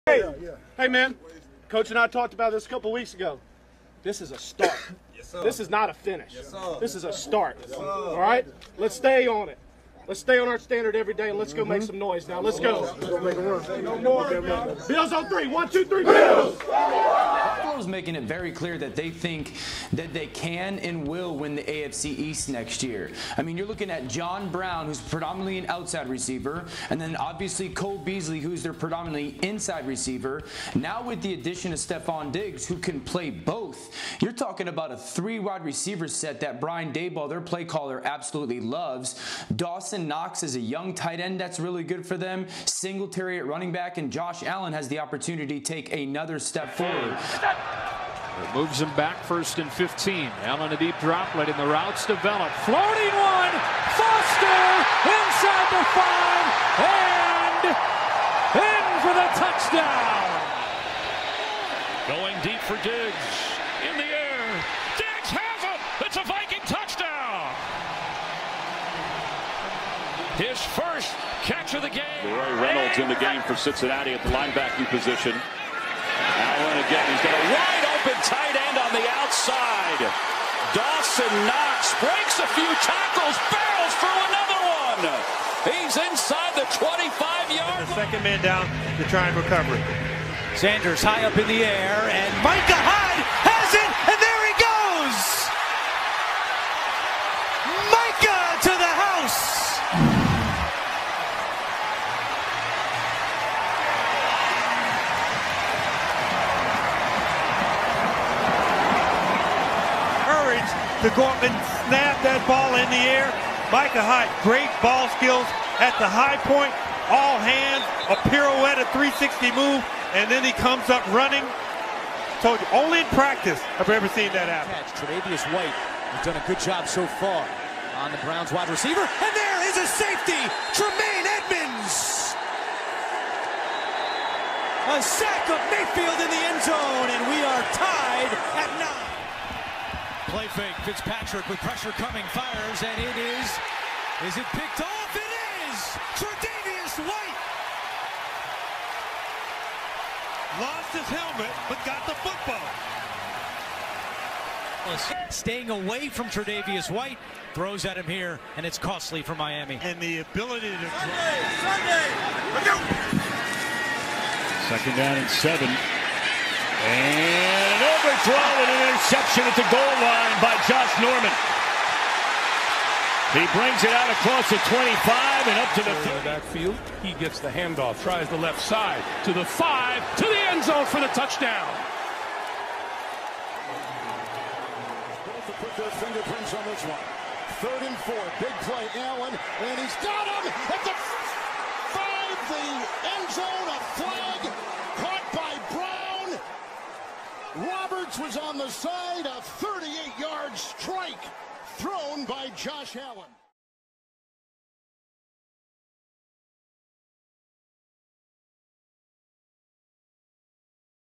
Buffalo Bills 2020-2021 Season Hype
Hype up video for the buffalo bills.